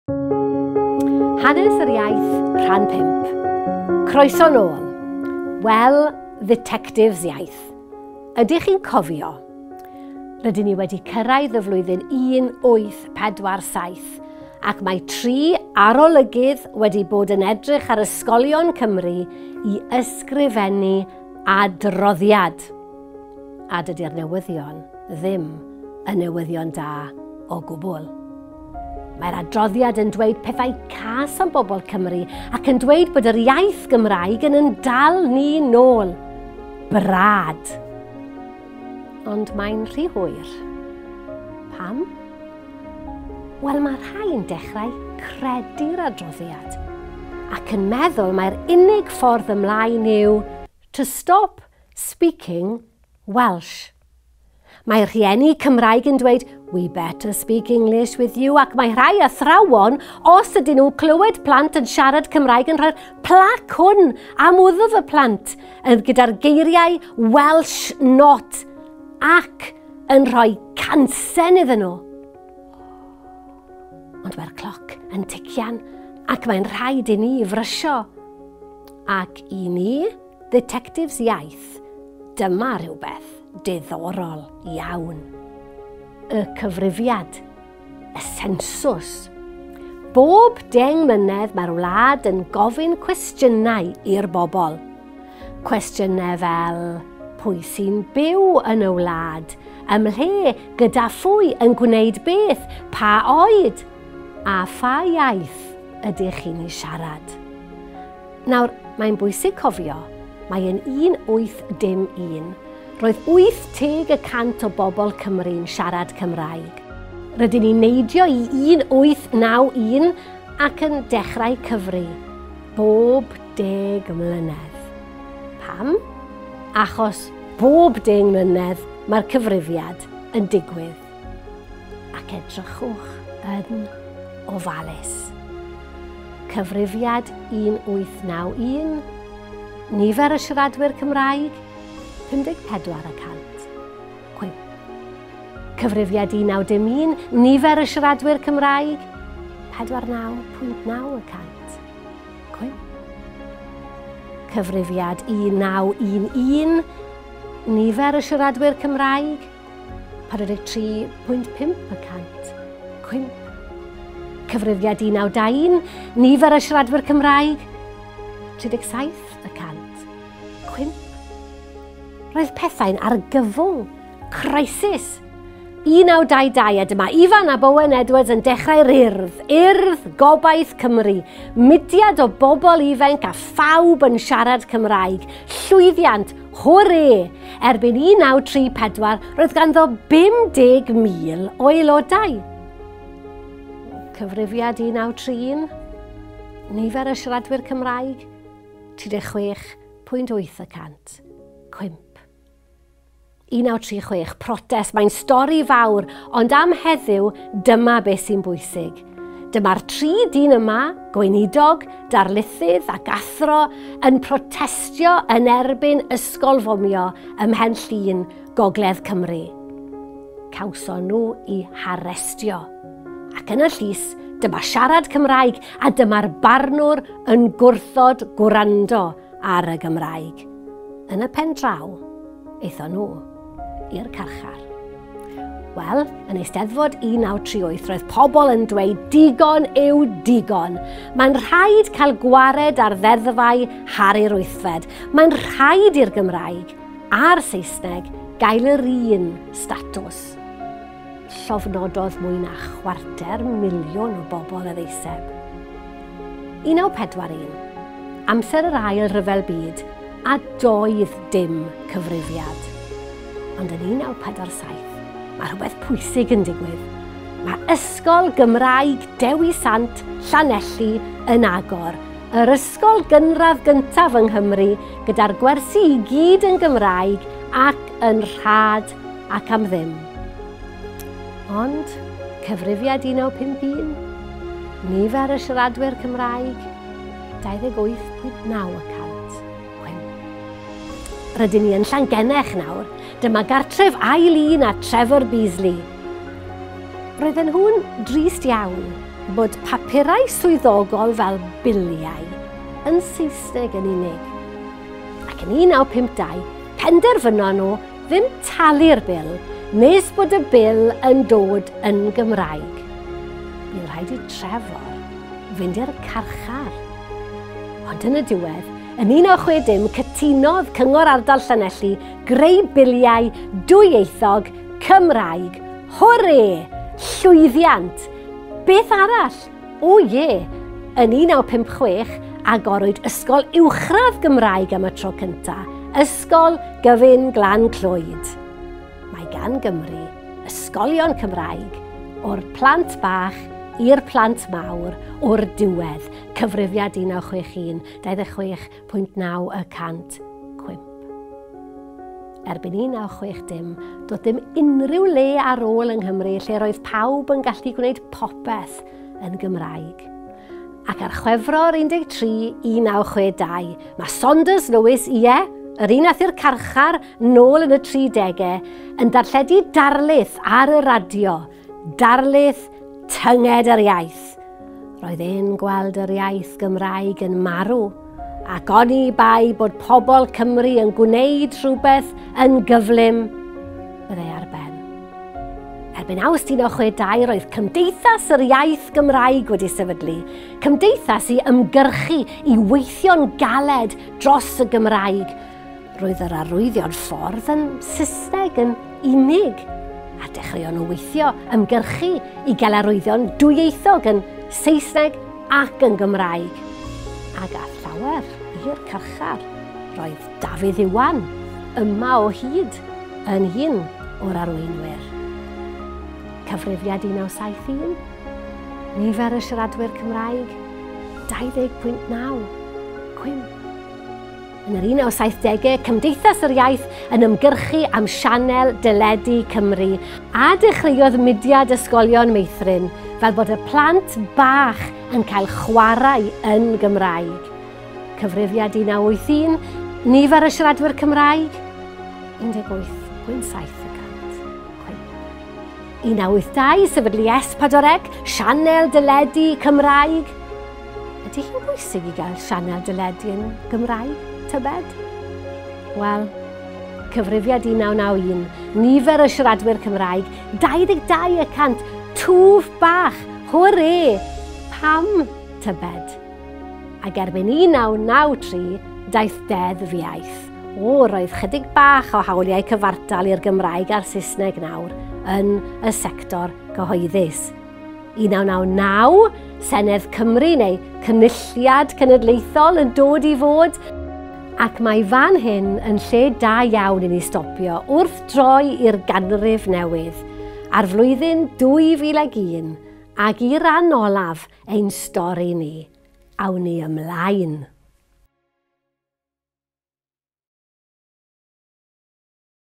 Hanes Yr Iaith (dysgwyr) Rhan 5
Dewch i olrhain hanes Cymru gyda Mererid Hopwood.

Come trace the history of Wales with Mererid Hopwood.